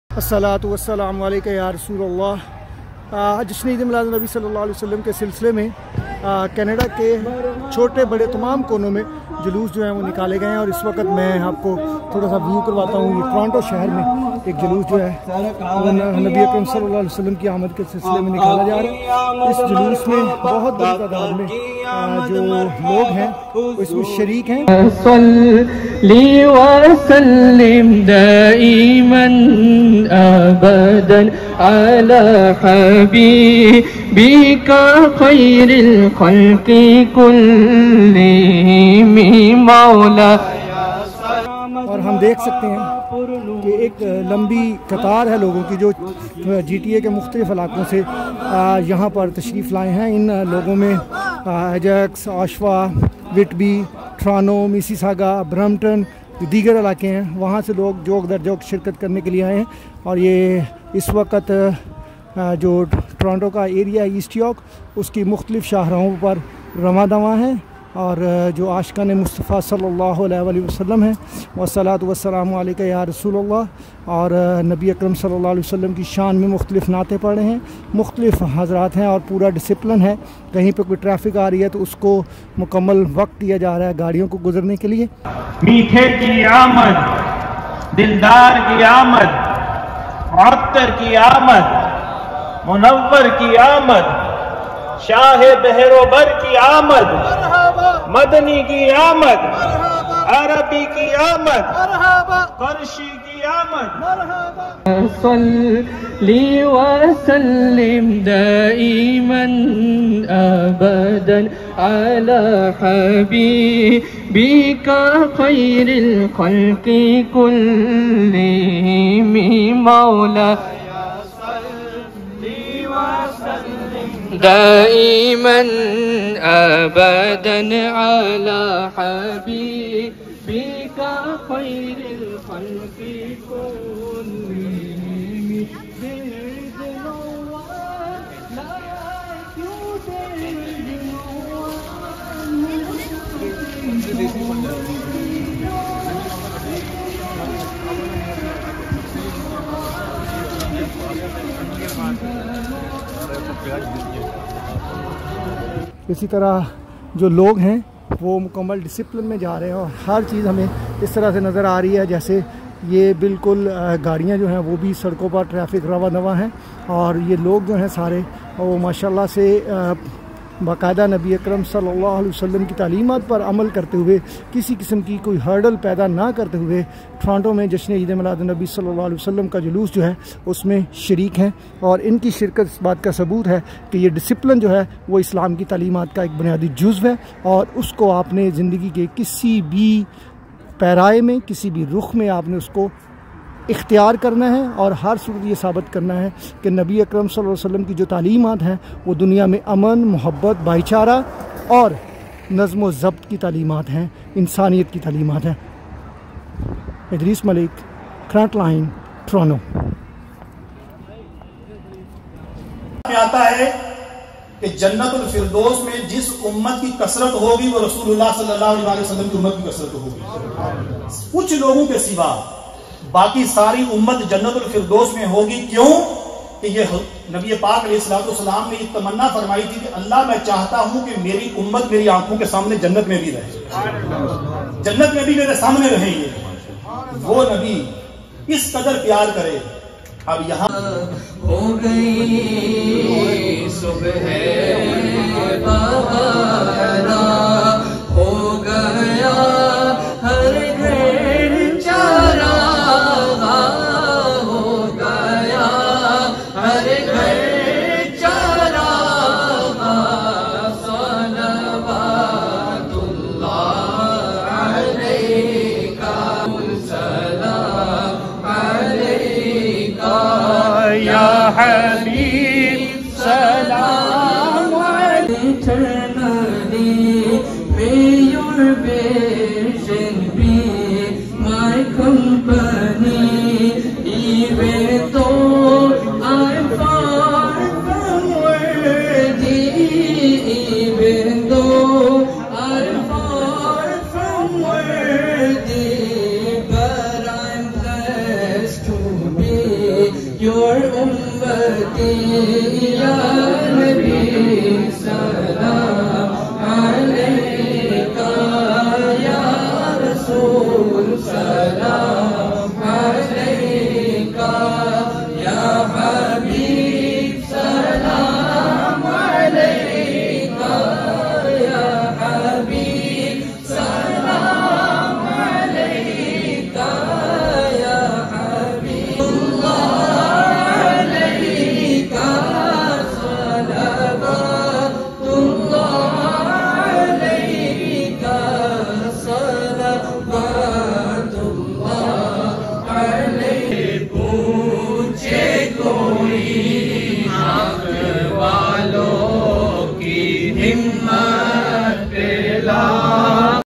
Eid Milad Rally in Canada موئے مبارک حضرت محمد صلی اللہ علیہ والہ وسلم کی زیارت
This video is about Eid Milad Julus in Toronto